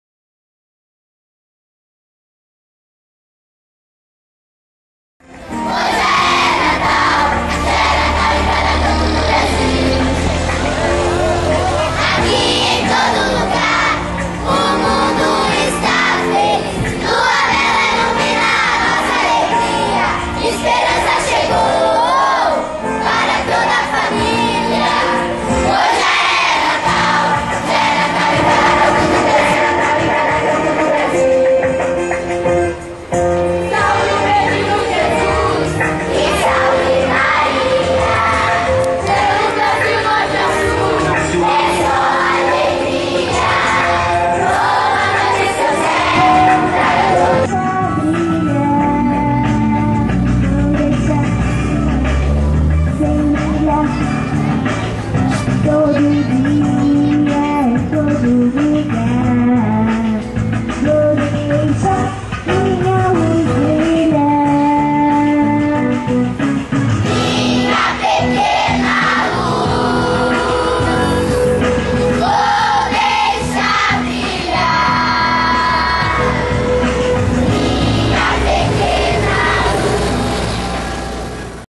CORAL DE NATAL
Apresentação Coral Escola Municipal Professor José Antônio Flygare/CAIC.
05 de dezembro de 2011.
Castro-Paraná.